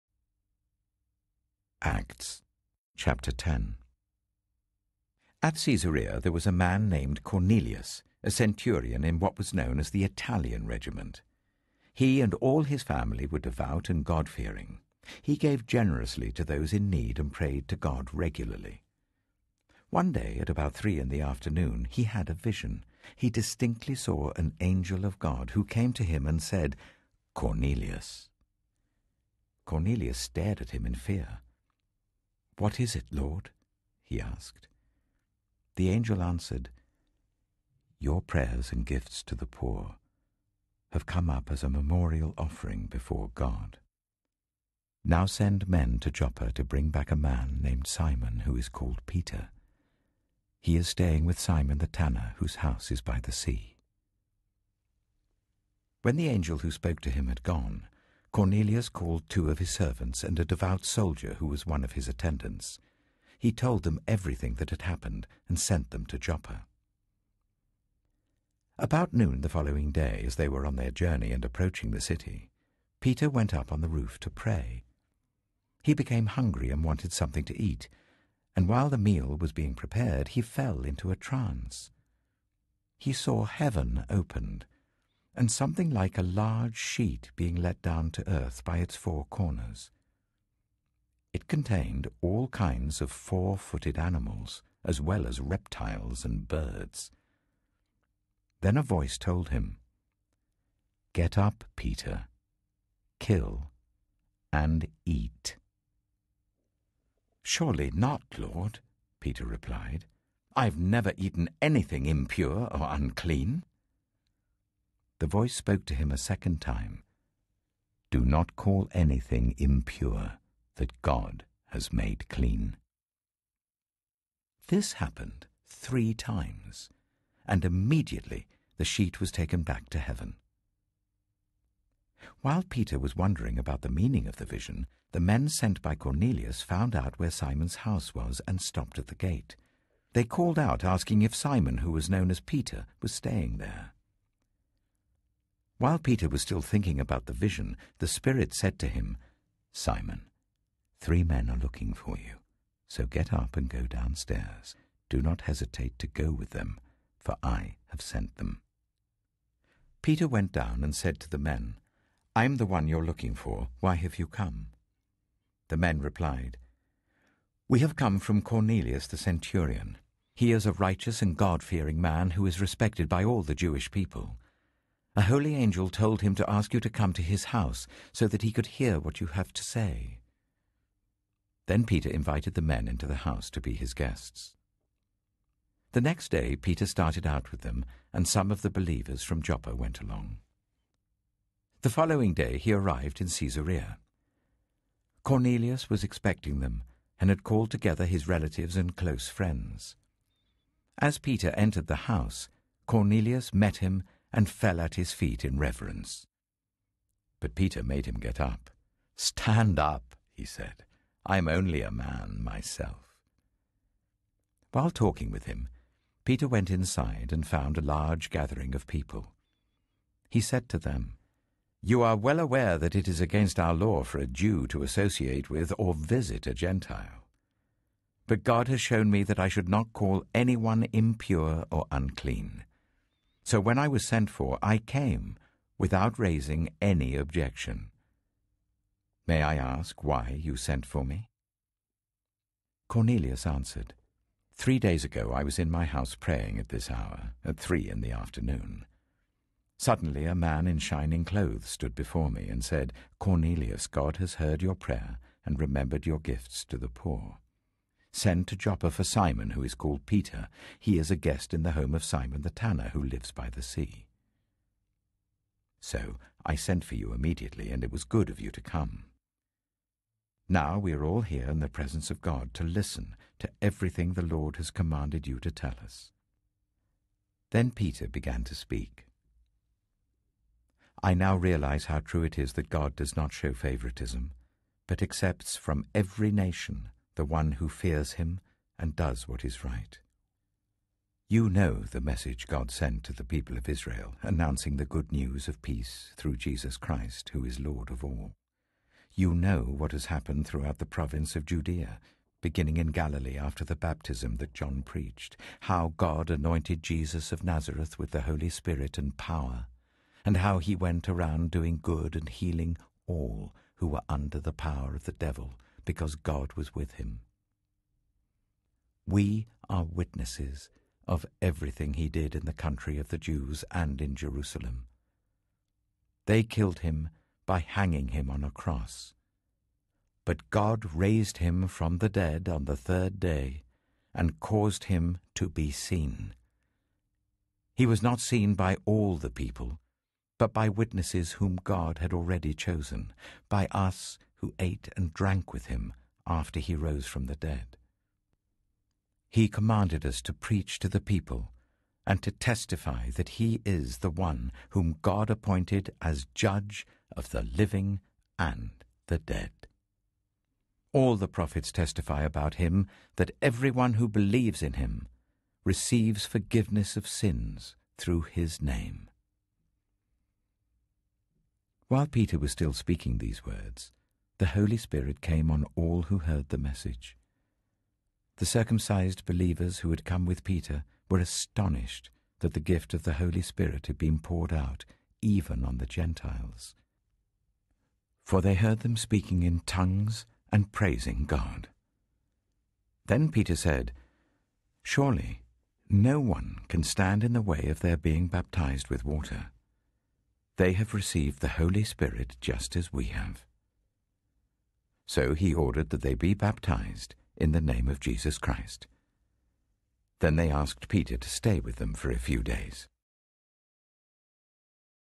David Suchet NIV Bible 1028 Acts 10
David Suchet reads Acts chapter 8 from the New International Version (NIV) of the Bible.